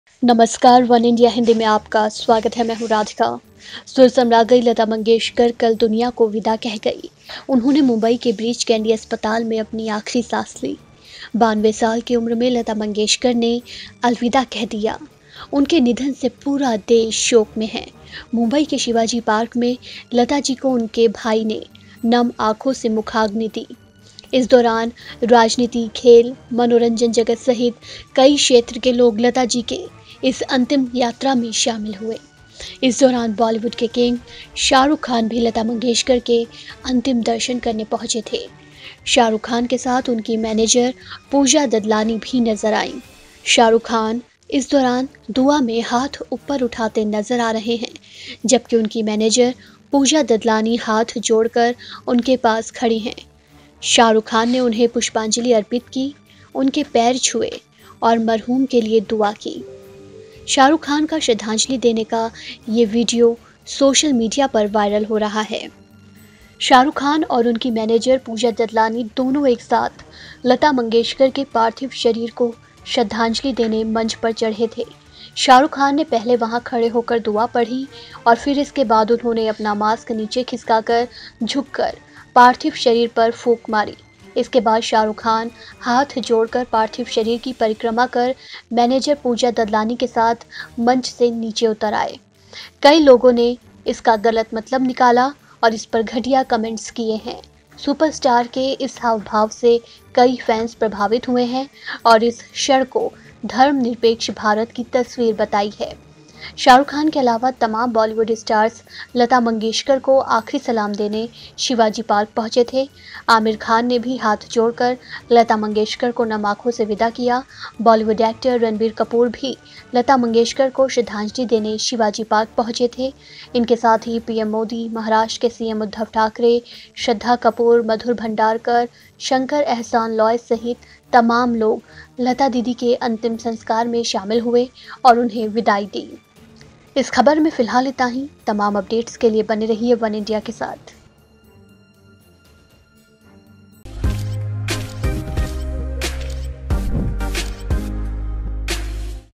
Shah Rukh Khan ने Lata Mangeshkar को दी श्रद्धांजलि, अंतिम संस्कार में की दुआ | SRK | वनइंडिया हिंदी
सुर साम्राज्ञी लता मंगेशकर  (Lata Mangeshkar ) दुनिया को विदा कह गईं। उन्होंने मुंबई के ब्रीच कैंडी अस्पताल   (Breach Candy Hospital in Mumbai) में आखिरी सांस ली। मुंबई के शिवाजी पार्क (Shivaji park) में लता जी को उनके भाई ने नम आंखों से मुखाग्नि दी गई। इस दौरान राजनीति, खेल, मनोरंजन जगत सहित कई क्षेत्र के लोग लता जी की इस अंतिम यात्रा (lata mangeshkar funeral) में शामिल हुए।  इस दौरान बॉलीवुड के किंग शाहरुख खान भी लता मंगेशकर के अंतिम दर्शन करने पहुंचे थे। शाहरुख खान (Shah Rukh Khan) के साथ उनकी मैनेजर पूजा ददलानी (Pooja Dadlani) भी नजर आईं।

melody queen Lata Mangeshkar bid farewell to the world. She breathed her last at Breach Candy Hospital in Mumbai. Lata ji was lit with moist eyes by her brother at Shivaji Park in Mumbai. During this, people from many fields including politics, sports, entertainment world participated in this last journey of Lata ji. During this, King of Bollywood Shahrukh Khan also came to pay the last respect to Lata Mangeshkar. His manager Pooja Dadlani was also seen with Shah Rukh Khan.